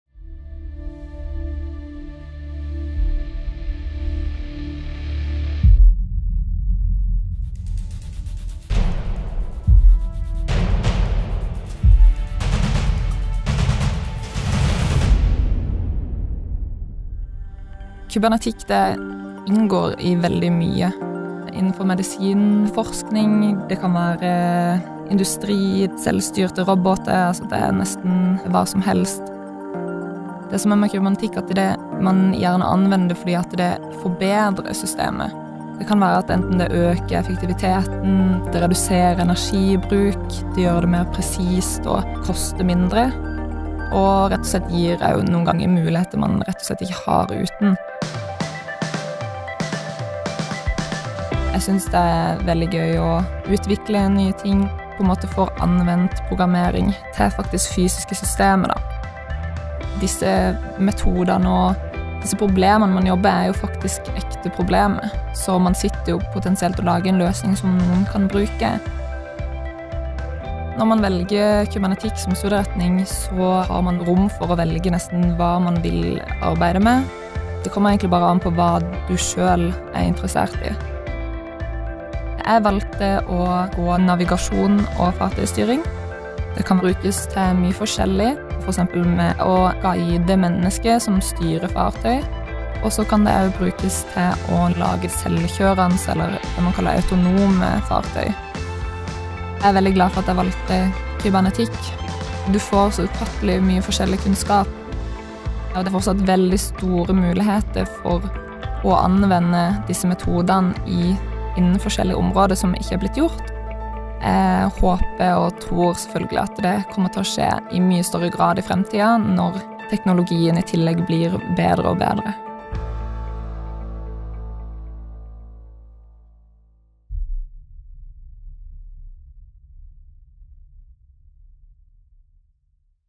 Master i kybernetikk og robotikk | NTNU
Vil du være med på å forbedre samfunnet vårt?

Kybernetikk og robotikk er studiet for deg som ønsker å jobbe med fremtidens teknologier, i en verden som blir stadig mer digitalisert, automatisert og robotisert. Med kybernetikken kan vi skape et miljøvennlig fremtidssamfunn med meningsfylte jobber og mer fritid. Kybernetikere utvikler teknologi for fremtidens arbeidsliv, og du vil lære deg verktøy for å håndtere en stadig mer kompleks verden.